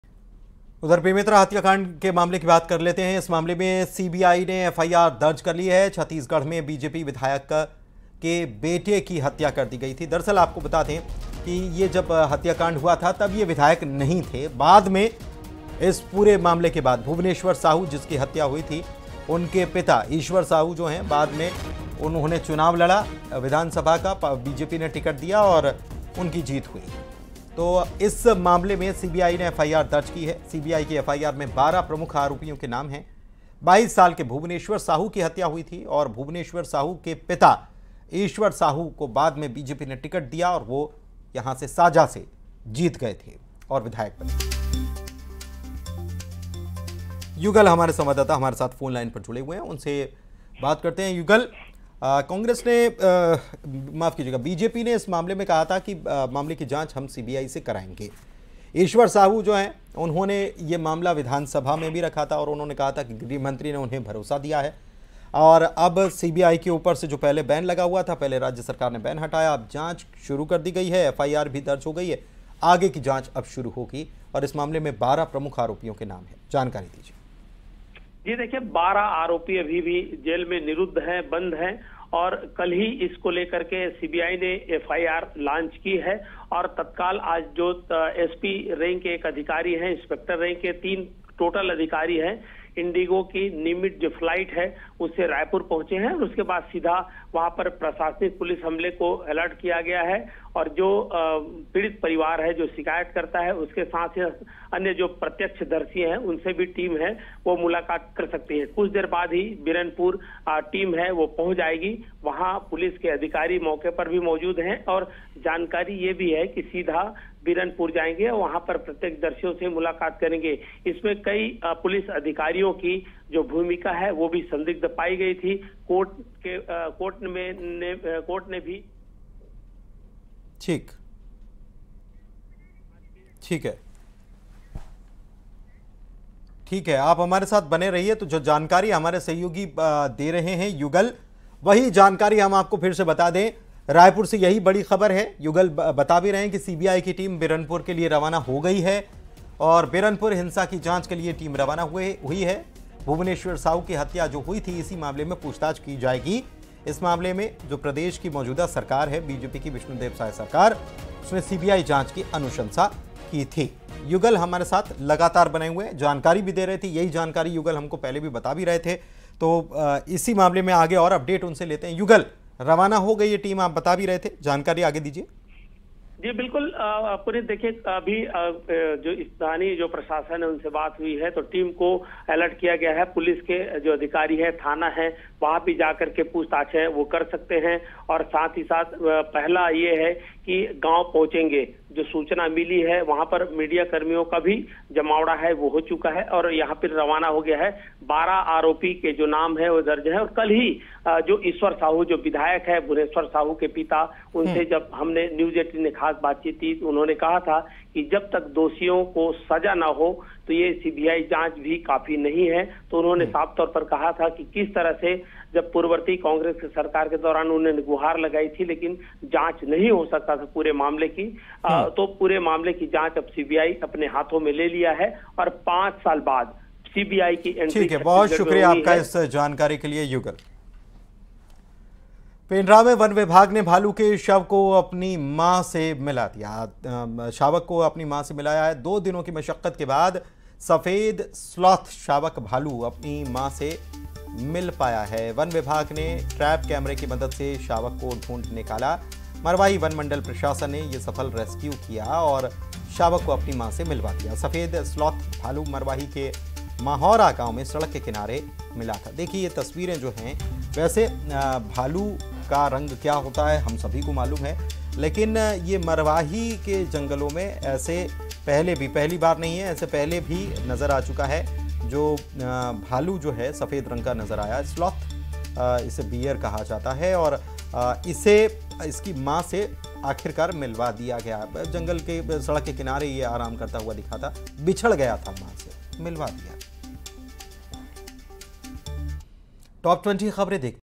Chhattisgarh News : सांप्रदायिक हत्याकांड में CBI ने दर्ज की FIR, आज आएगी CBI की Team |Bemetara News
बेमेतरा हत्याकांड में CBI ने दर्ज की FIR, जिसमें बीजेपी विधायक के बेटे की हुई थी हत्या